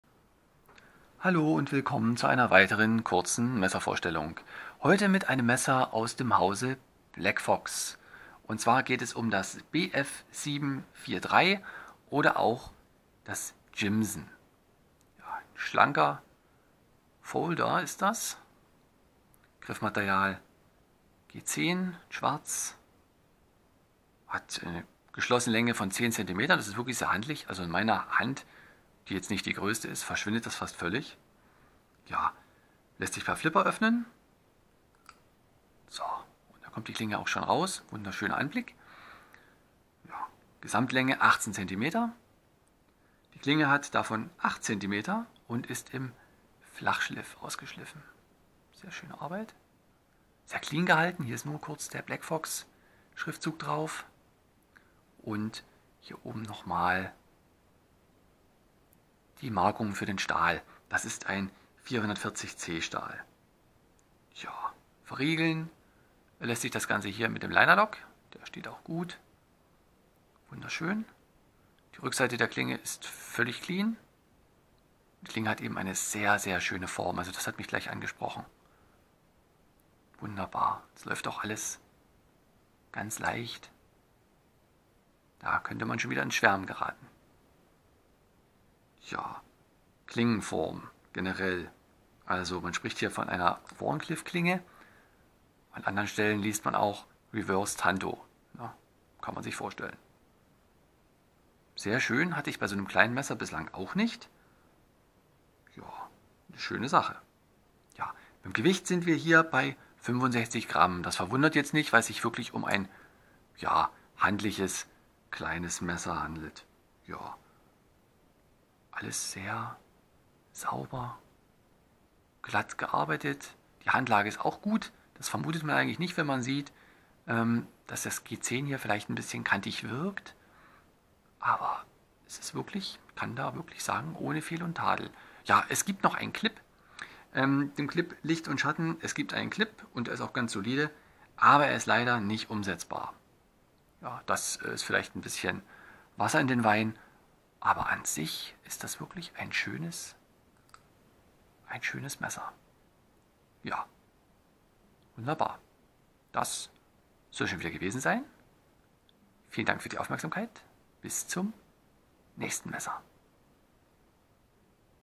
BlackFox Jimson - schlicht im Auftritt und dennoch überzeugend
In dieser Messervorstellung geht es um das Jimson der italienischen Firma BlackFox. Das schlanke Design und die Klingenform haben mich sofort überzeugt, zumal die Kombination aus G10-Griffmaterial und 440C-Stahl für eine solide Konstruktion bürgt. Der Lauf ist butterweich, die Verarbeitung ohne Tadel und dies alles im Budget-Bereich - das geht schon mal...

Wie immer, keine Werbung, nur die Schilderung persönlicher Eindücke. Gute Unterhaltung!